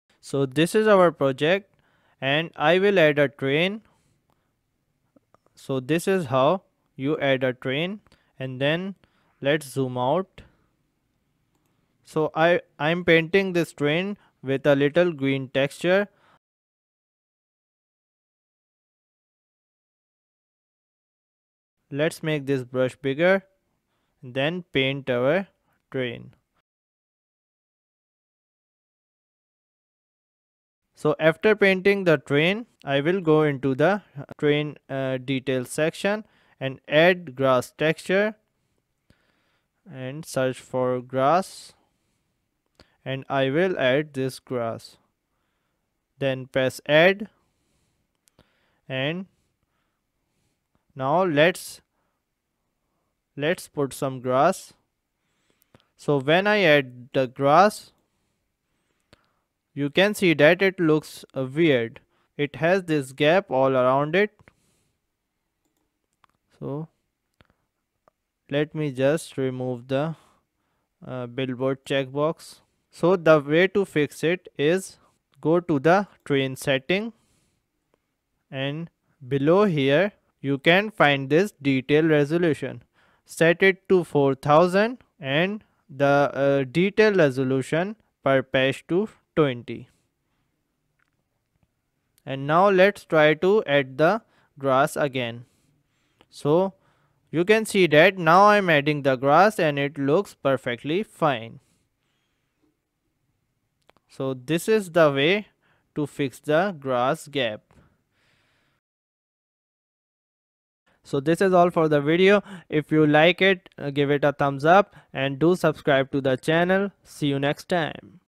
Unity Grass Gap Problem Resolved | Grass Fix for Unity Engine
video is about grass gap problem when adding grass on terrain using the add details section. there is space between the grass and the empty space looks pretty weird.
grass details missing, can't paint thick grass gap between grass.

my channel is not about game development but i thought this video would help many so here it is!